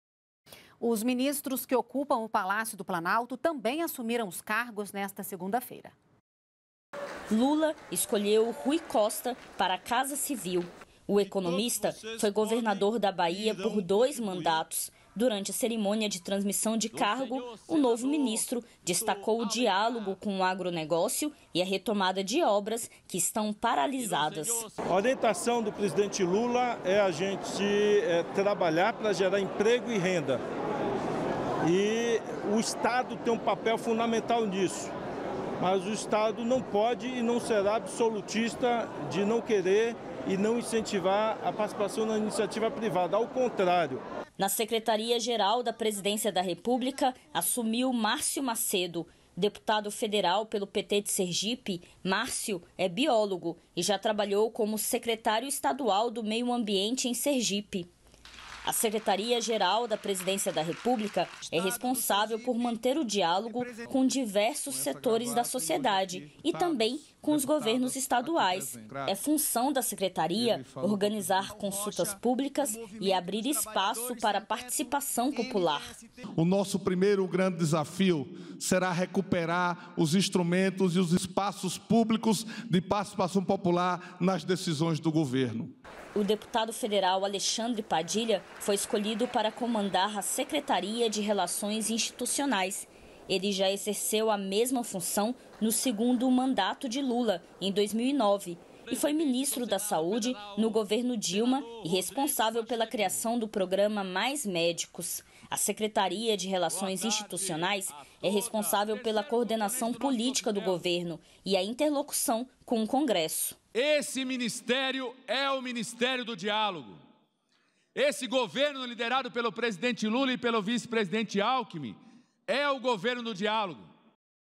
Governo | Posse no Planalto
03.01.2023 - Os ministros que ocupam o Palácio do Planalto assumiram os cargos nessa segunda-feira.